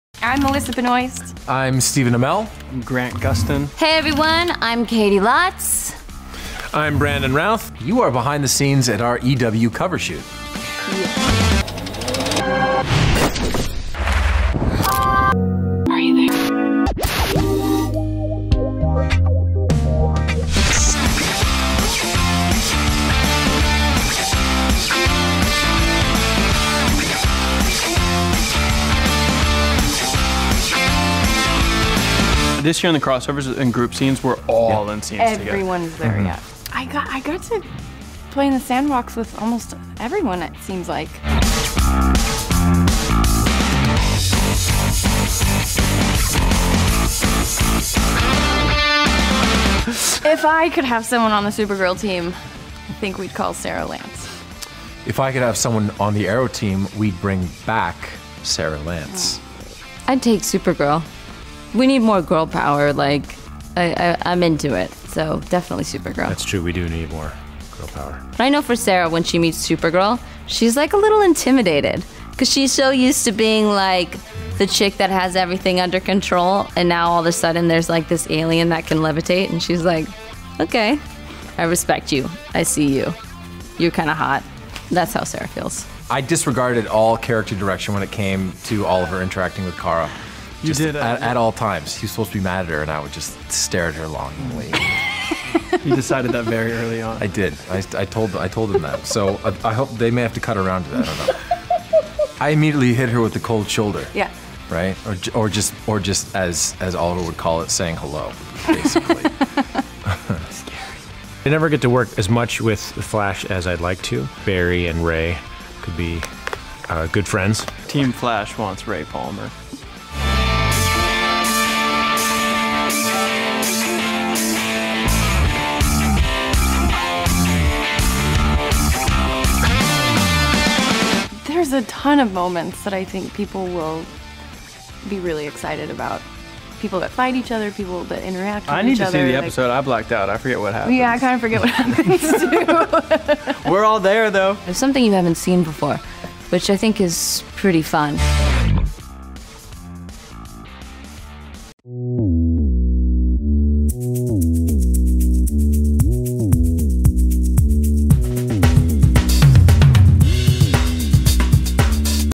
CW Superheroes Crossover: Behind The Scenes | Cover Shoot | Entertainment Weekly
Go behind the scenes of our shoot with CW superheroes Melissa Benoist, Grant Gustin, Stephen Amell, Caity Lotz and Brandon Routh.